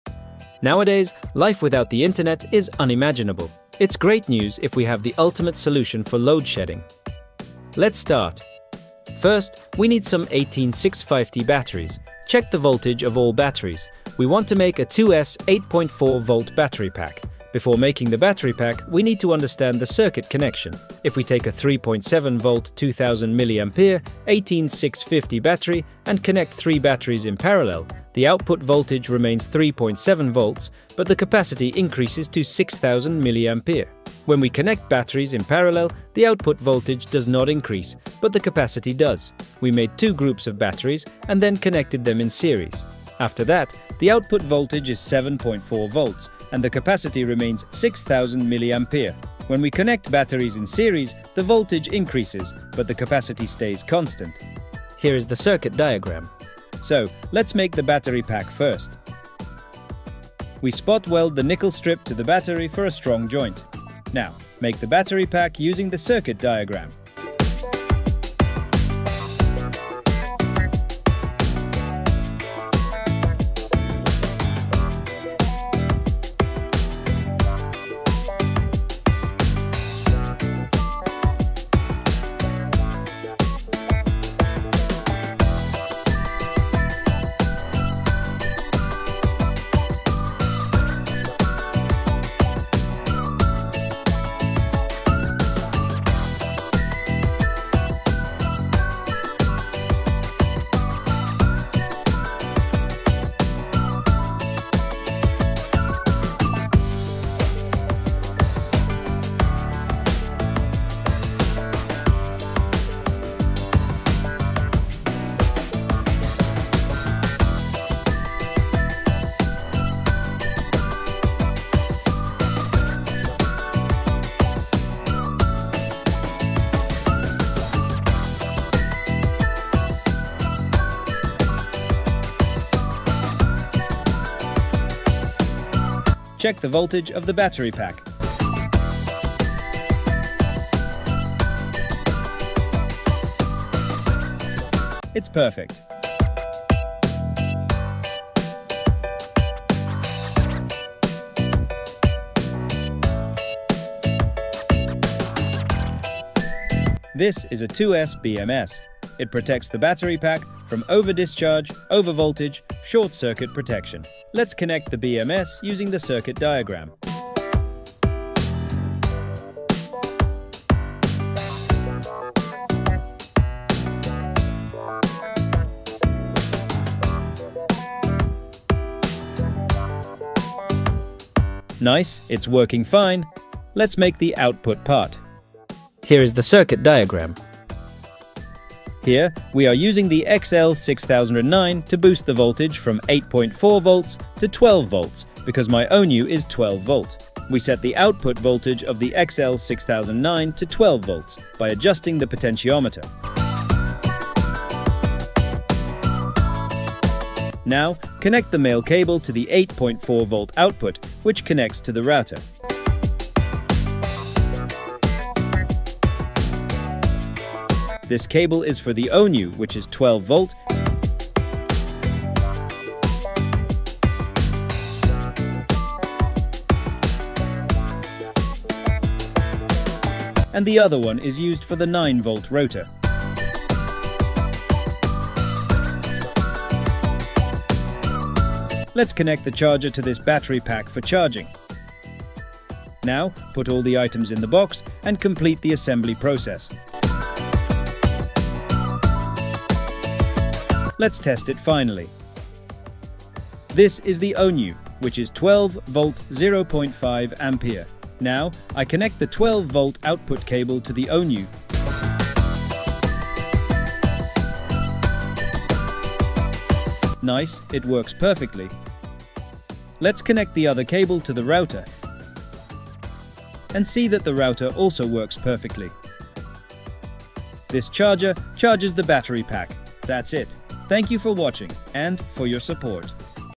Easy "DIY Mini UPS" Project Using 18650 Batteries | CUBIT
Easy DIY Mini UPS Project Using 18650 Batteries

In this video, I'll show you how to build a DIY Mini Uninterruptible Power Supply (UPS) using 18650 batteries. This compact UPS is perfect for keeping your small electronic devices, like WiFi routers and CCTV cameras, powered during outages. Follow along with this step-by-step guide and learn how to assemble your own reliable backup power source at home.

Materials Needed:
- 18650 Batteries
- 8.4V Battery Pack
- Battery Management System (BMS) 2S 8.4V
- Spot Welder/Soldering Kit
- Power Management Module
- Connecting Wires

Steps:
1. Assemble the 18650 Battery Pack: Learn how to create a 18650 battery pack, either 12V or 8.4V, suitable for your needs.
2. Install the BMS: Understand how to properly connect and install a BMS 3S 12V or 2S BMS to ensure safe and efficient battery management.
3. Soldering Components: Follow the detailed soldering process to securely connect all components.
4. Build the Mini UPS: Step-by-step instructions to assemble the mini UPS, perfect for use as a UPS for WiFi routers, CCTV, or other small devices.
5. Testing and Usage: Final steps to test your DIY mini UPS and ensure it works seamlessly to keep your devices powered during outages.

Timestamps:

00:00 Introduction
00:21 Circuit Connection
00:58 Battery Pack Build
02:37 Installing 2s BMS
03:05 Ups Circuit Connection
04:03 Assemble All The Parts
04::10 Testing and Usage

Why Build a DIY Mini UPS?
Creating your own mini UPS at home not only saves money but also provides a customized solution for your specific needs. Whether you need a router UPS for uninterrupted internet or a compact UPS for your security system, this project is perfect for DIY electronics enthusiasts.

*CUBIT:* We Deliver Our Experience!